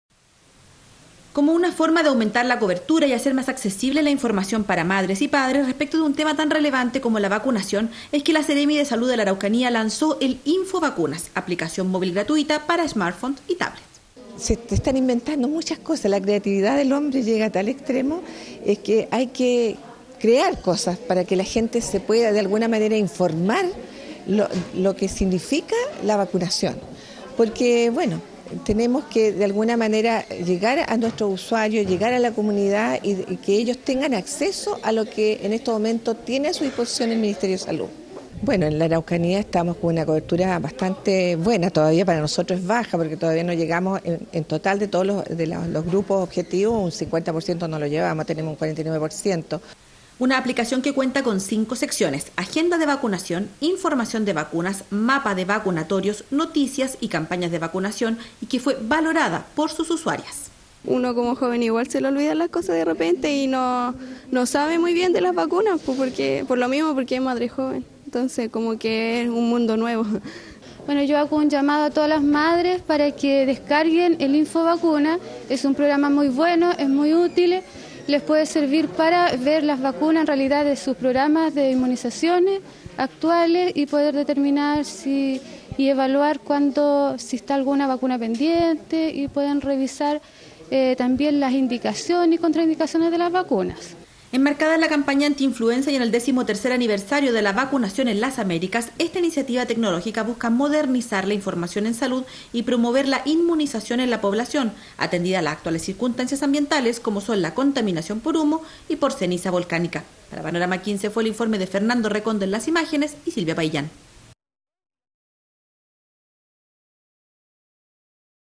APP PARA VACUNACIÓN EN ARAUCANÍA
Una novedosa aplicación móvil que se registró en la Araucanía permite a sus usuarios estar al tanto de los procesos de vacunación de toda su familia en un solo clic. No sólo es una especie de agenda virtual sino que también una útil herramienta para conocer y prevenir enfermedades principalmente en niños. Silvia Paillán nos inyecta la siguiente nota